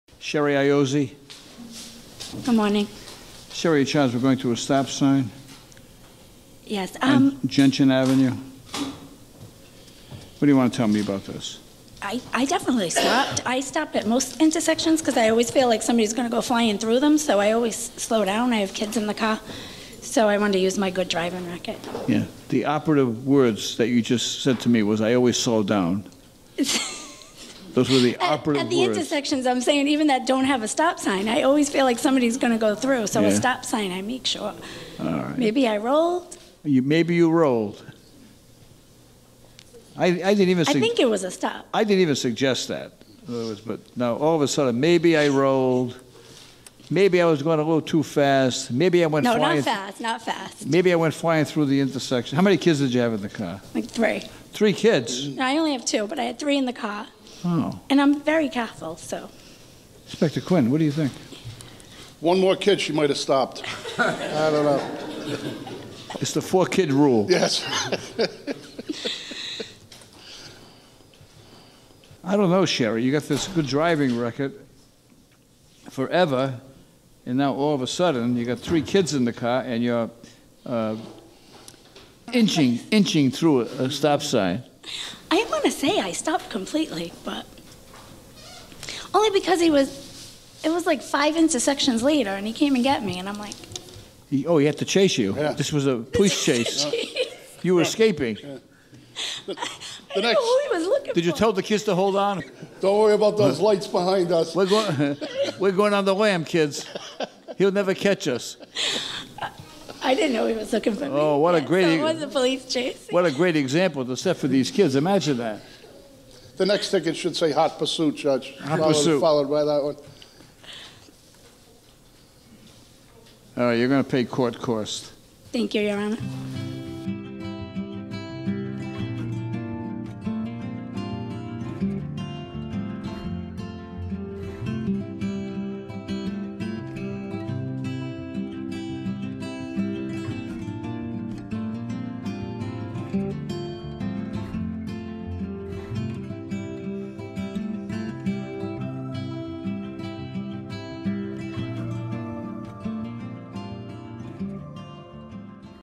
The "Four Kid" Rule
A motorist has a unique excuse for rolling through a stop sign.